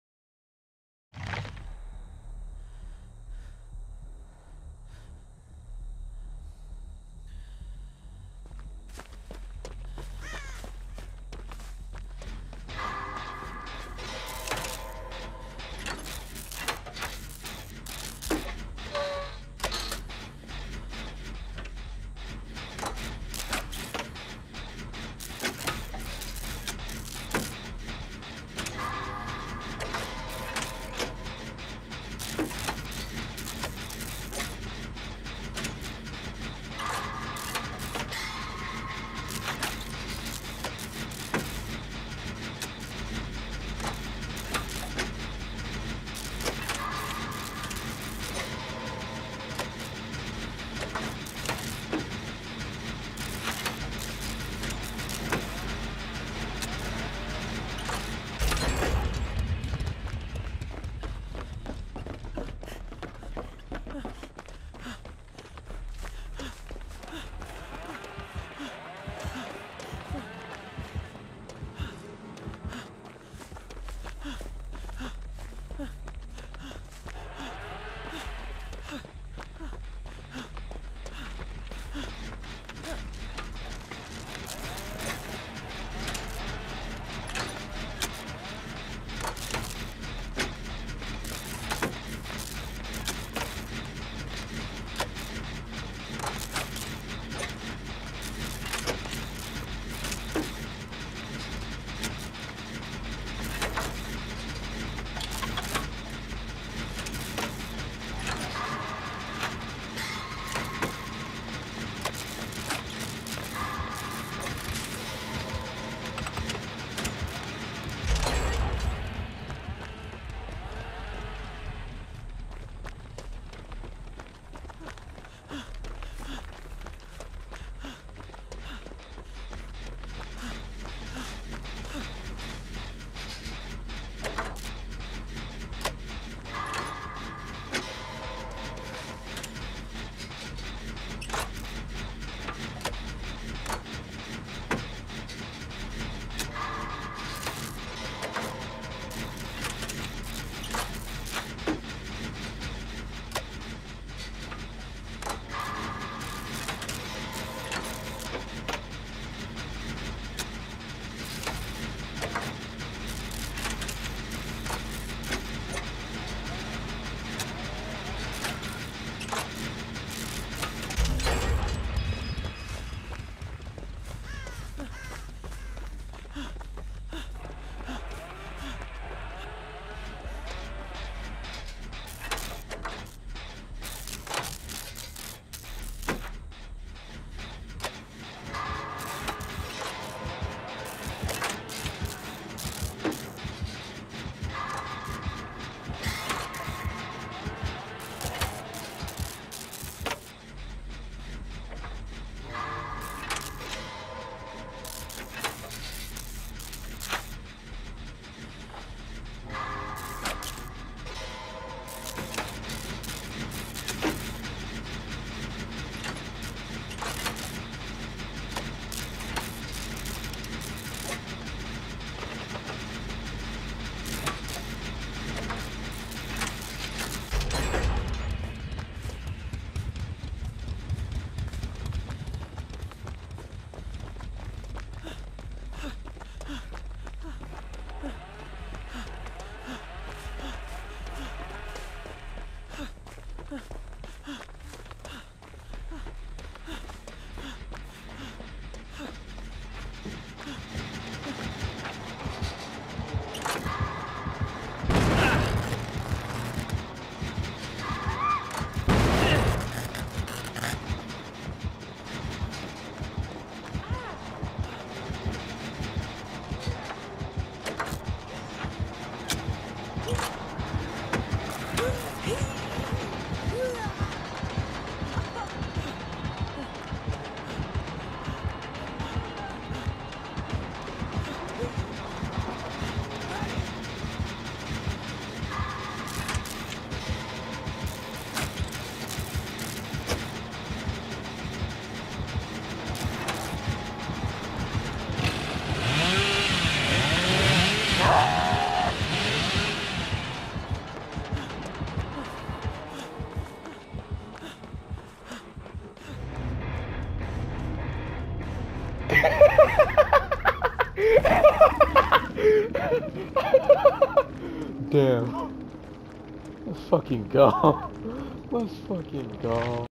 POV: your teamate looped the killer. Killer DCs
Feels bad moment